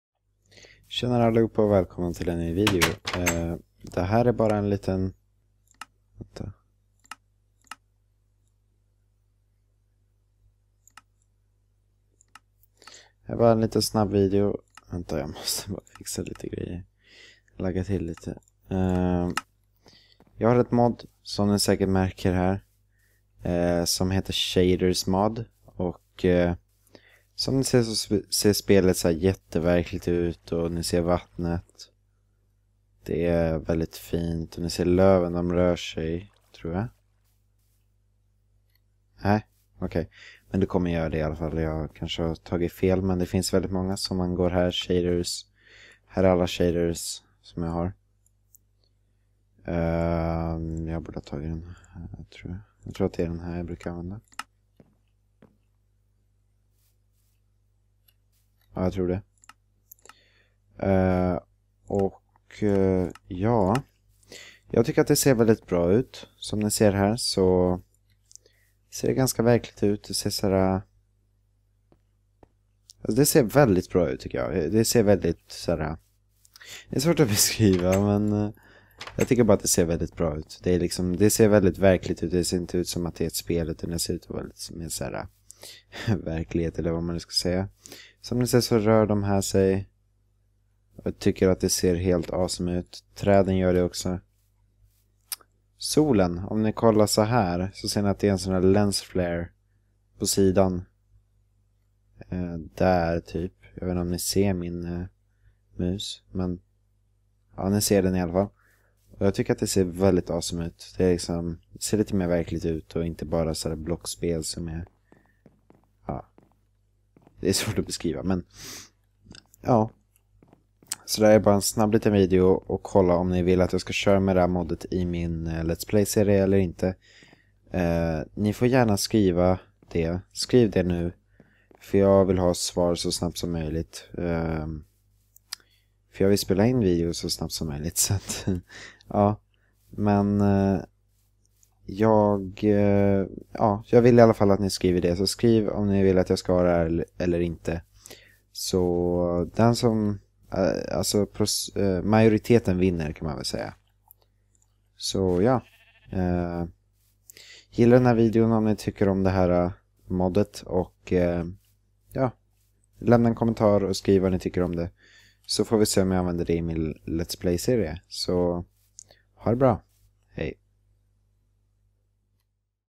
SHADERS MOD ELLER INTE?
Tjenare allihopa! Det här är en liten speciell video där jag vill att ni skriver om ni vill att jag ska ha Shaders mod eller inte! Skriv NU! :)